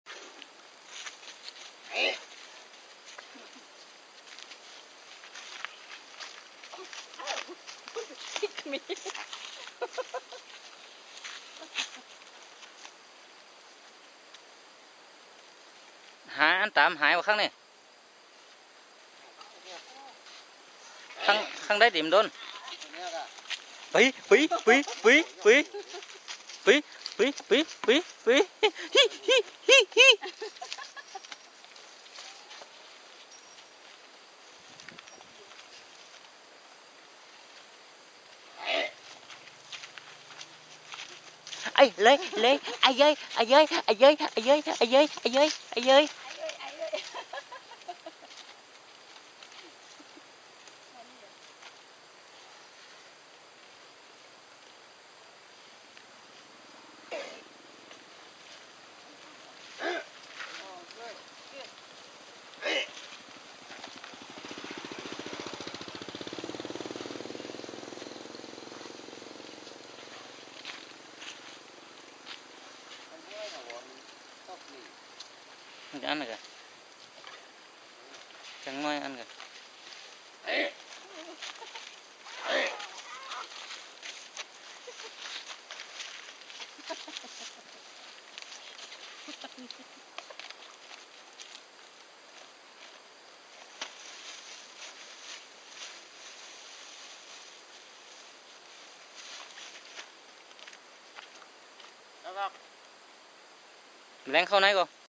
baby elephant 53days old playing.part 2ゾウの赤ちゃん 遊び パート２
Wanna's boy is very naughty and likes to play with people and he always goes back to mama . on 22nd September 2011. Thai elephant conservation center . blog ワナ―の男の赤ちゃんはとても腕白で人と遊ぶのが好き。でもいつもママのところへもどります。２０１１年９月２２日　タイゾウ保護センターにて。ブログ